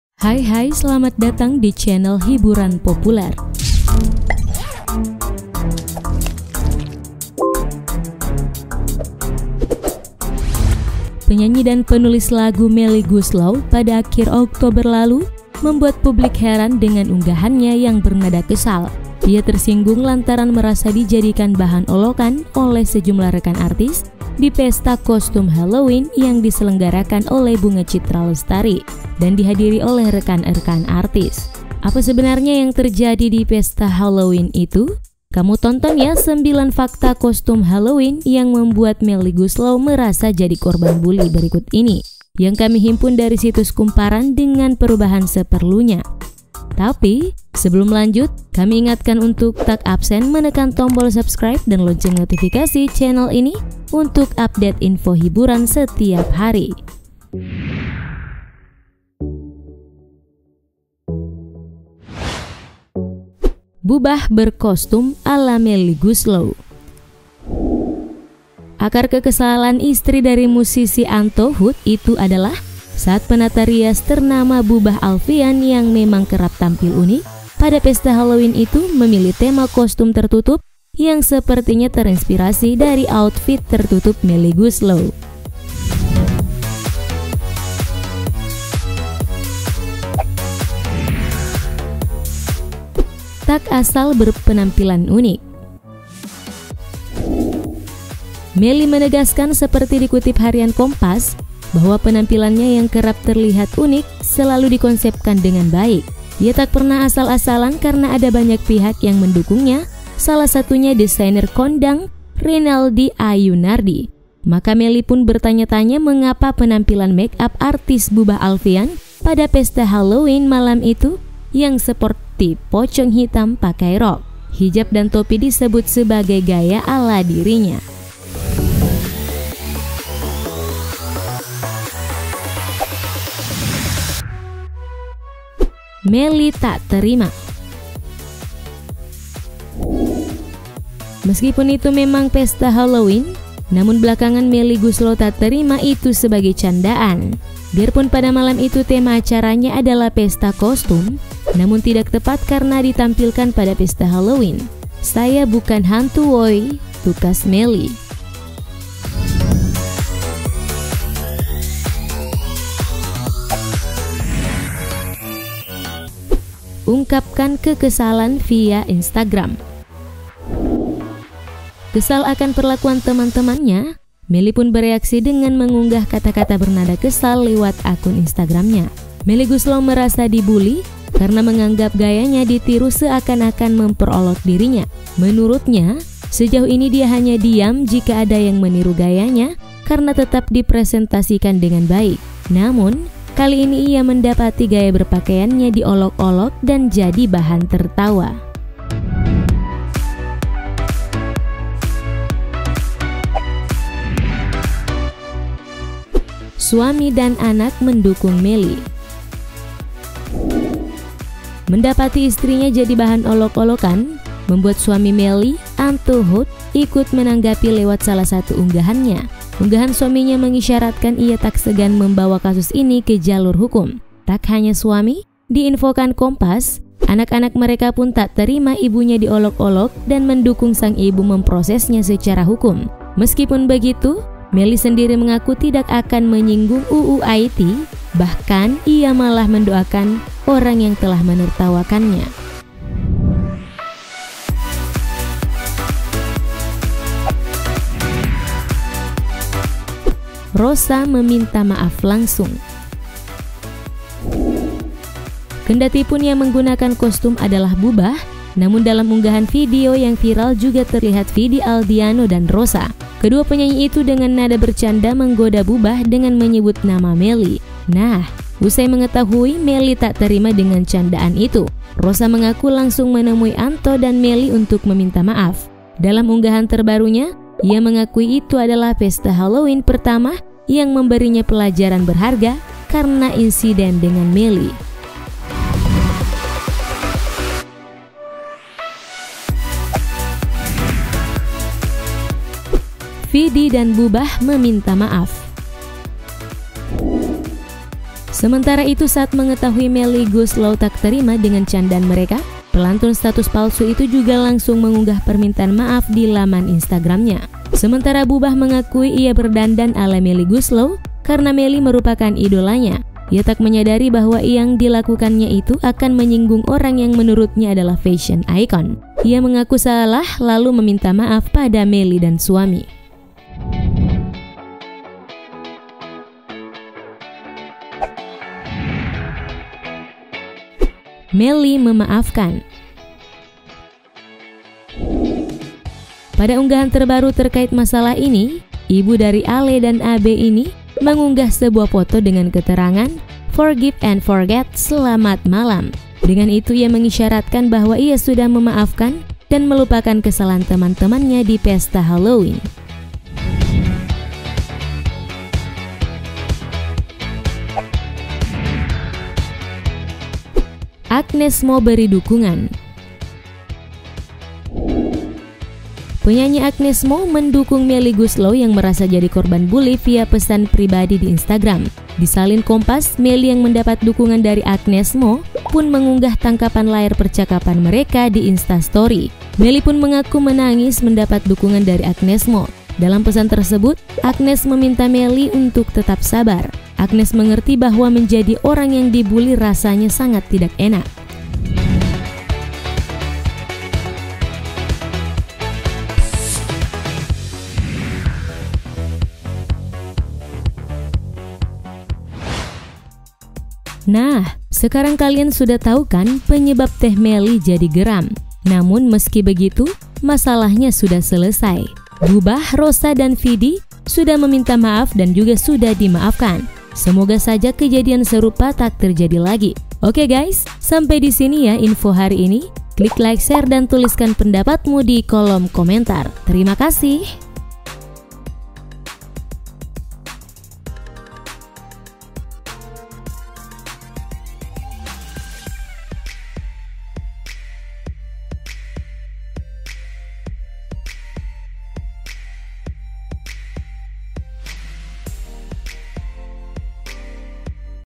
Ketika Bercanda Kelewat Batas! Melly Goeslaw Marah Besar Jadi Bahan Tertawaan di Pesta Halloween
Penyanyi dan penulis lagu Melly Goeslaw pada akhir Oktober lalu membuat publik heran dengan unggahannya yang bernada kesal. Ia tersinggung, lantaran merasa dijadikan bahan olokan oleh sejumlah rekan artis di pesta kostum Halloween yang diselenggarakan oleh Bunga Citra Lestari dan dihadiri oleh rekan-rekan artis. Apa sebenarnya yang terjadi di pesta Halloween itu? Kamu tonton ya 9 Fakta Kostum Halloween yang Membuat Melly Goeslaw Merasa Jadi Korban Buli berikut ini.